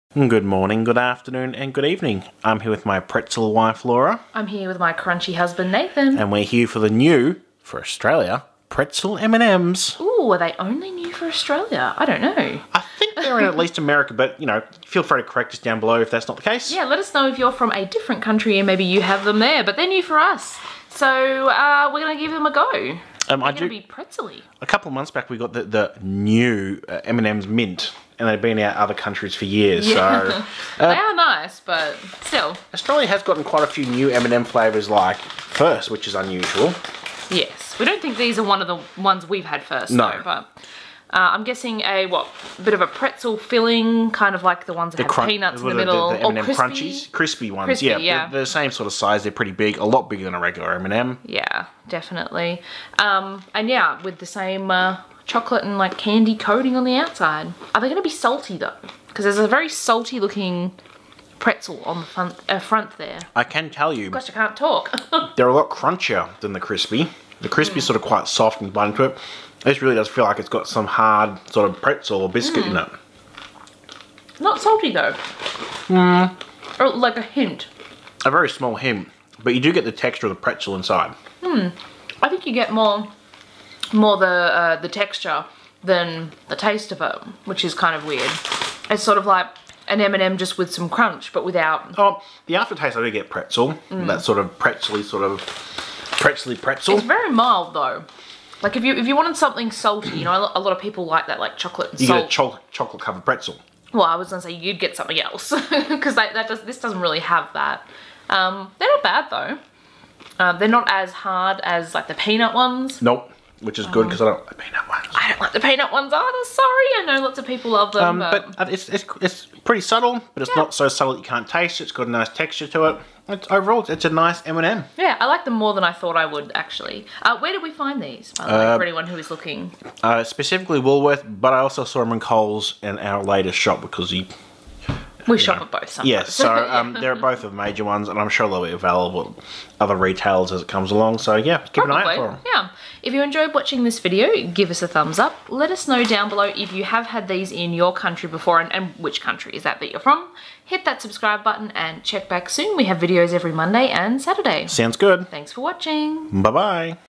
Pretzel M&M's Taste Test & Review! | Birdew Reviews
We taste test and review Pretzel M&M's new in Australia!